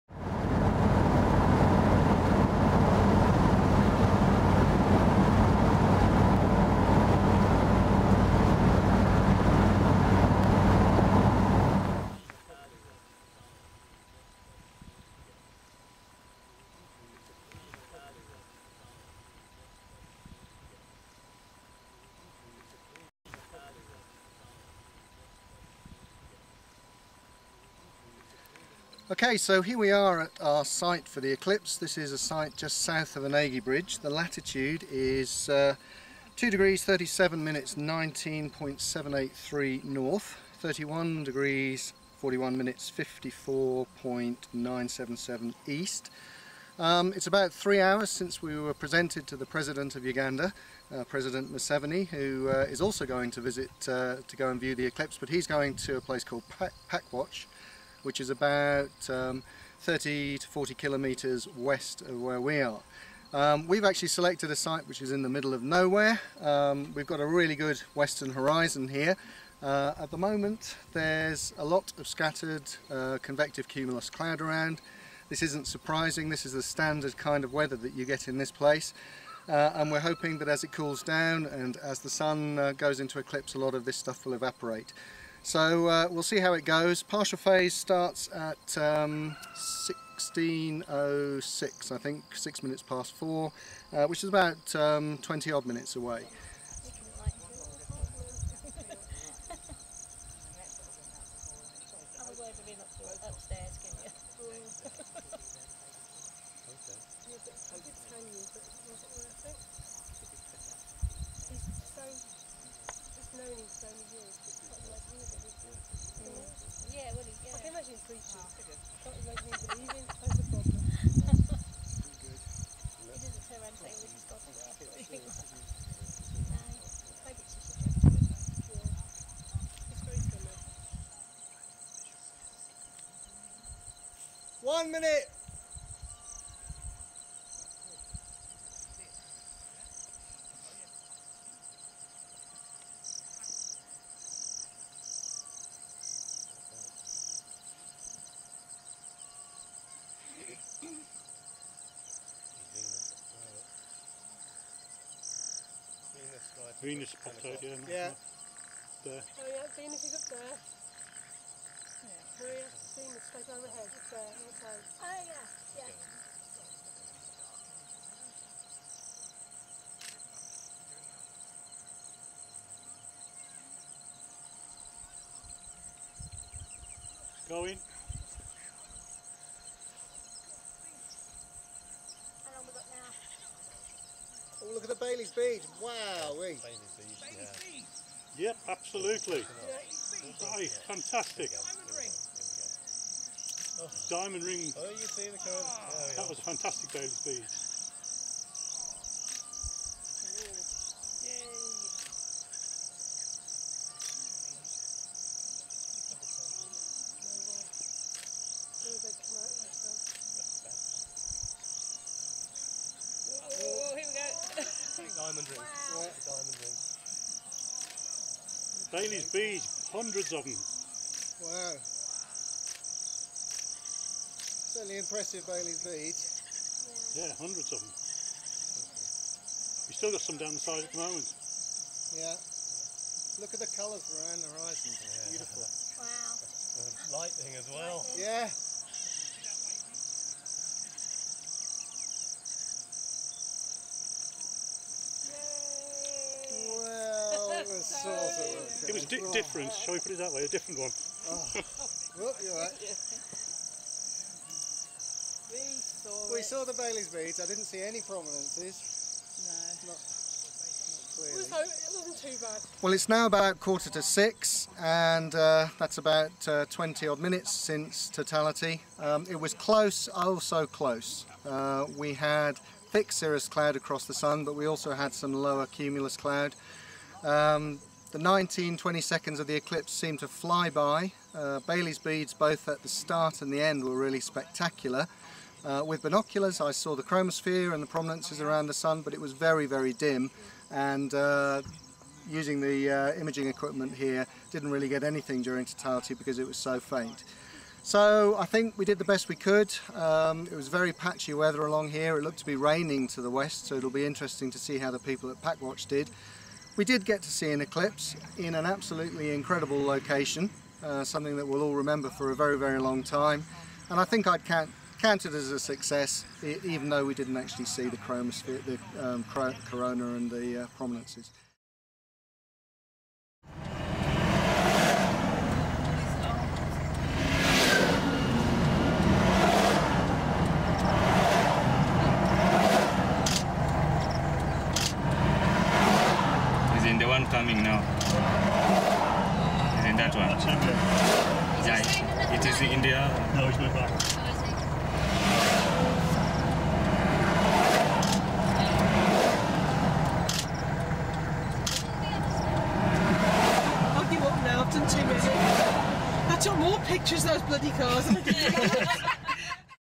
Total Solar Eclipse from Uganda
Edited HD video of the 2013 November 3 TSE from northern Uganda shot using a Panasonic TM700 and a Canon EOS550D.